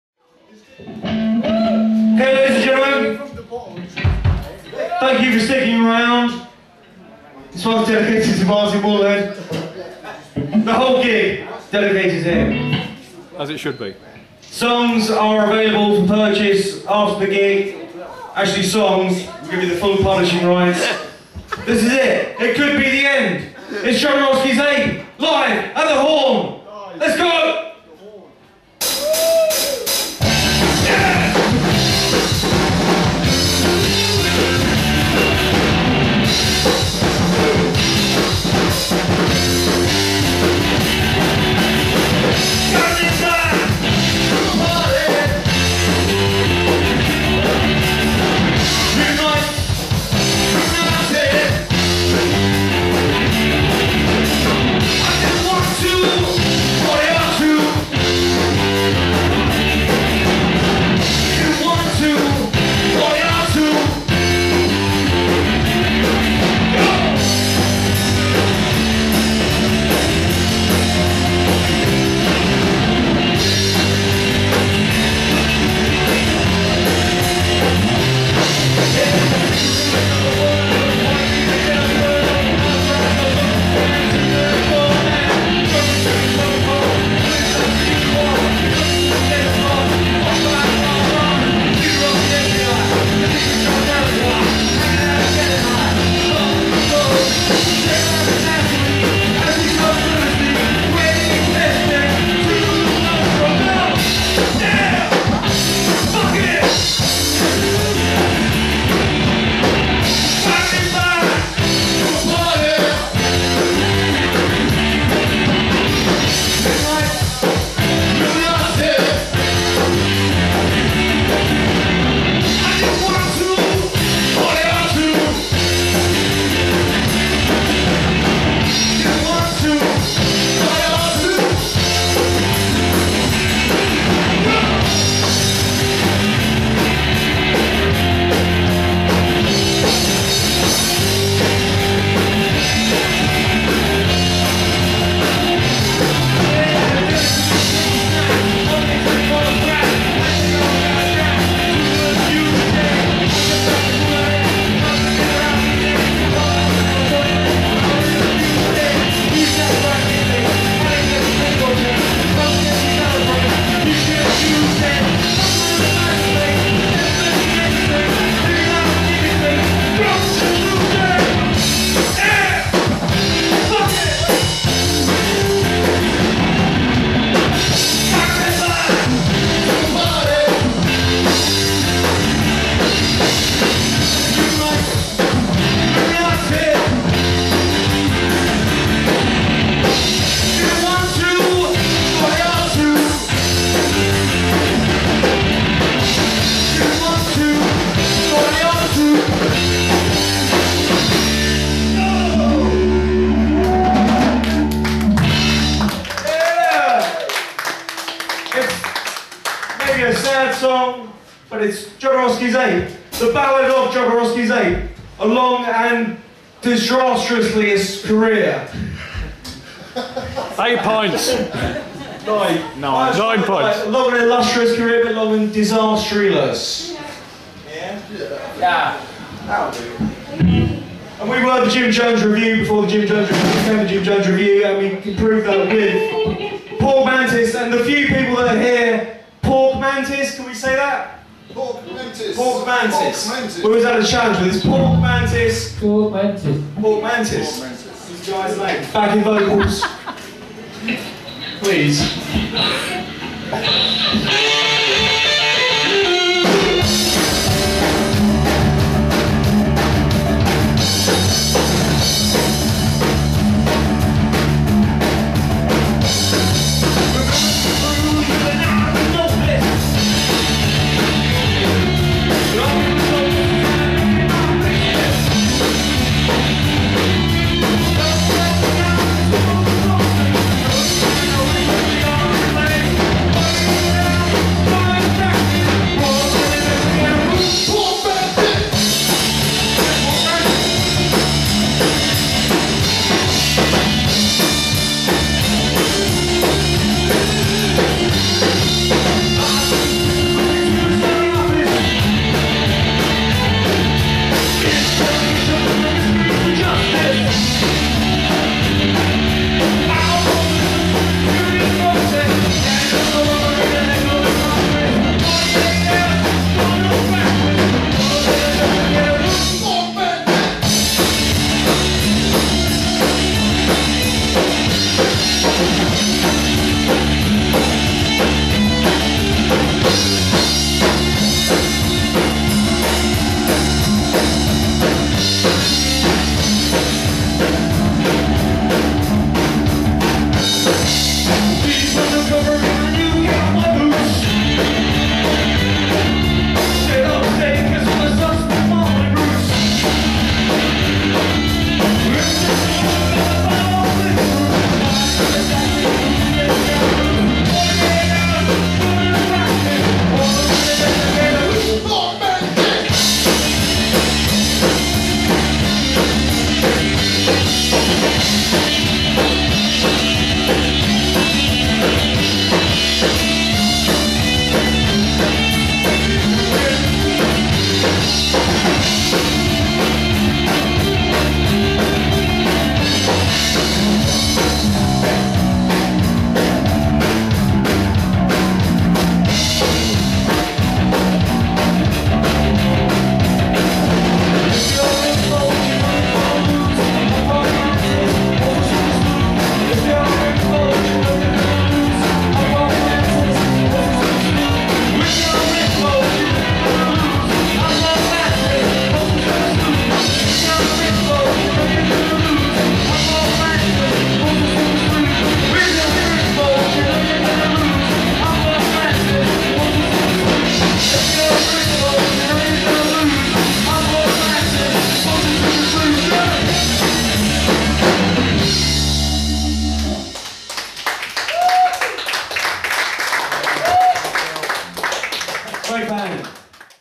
Jodorowsky's Ape, The Horn Reborn, St Albans, 30/09/10 : Part 1
Part 1 of not-sure-how-many-yet, as they will be edited and uploaded as and when the suckhole computer permits.

On the 30th September 2010 Jodorowsky's Ape played at The Horn Reborn in St Albans.

It was going to be their final gig, despite the fact they were advertized as appearing at the same venue a few months later.   We didn't believe it was their last gig.   Paul kept insisting it was the last gig they would do, but then he often does that.

Once again -  compressed within an inch of death for uploading and then compressed again by MyScreen  - we present something for the one subscriber to this channel and for Paul's ego: a visual record of possibly the last performance from Jodorowsky's Ape.

The gig was captured on a Handy Sonycam, and the footage is shaky at times as those things are a pain to hold steady for a long period of time.  I had to stand a fair distance from the stage to avoid the sound distorting more than the Daily Mail.